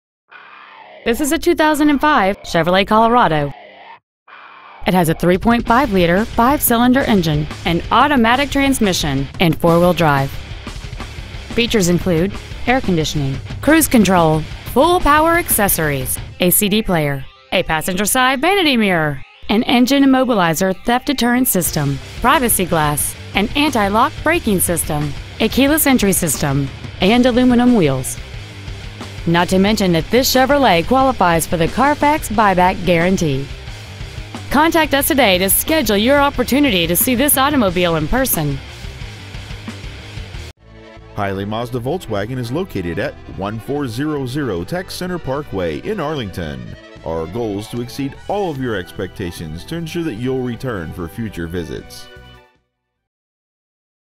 Pre-Owned 2005 Chevrolet Colorado Arlington TX 76014
Year : 2005
 Make : Chevrolet
 Model : Colorado
 Engine : 3.5L 5 cyl.
 Trans . : Automatic
 Exterior : Wheatland Yellow
 Miles : 93,152
 Interior : Sport Pewter w/Deluxe Cloth Seat Trim
 Stock : V47350A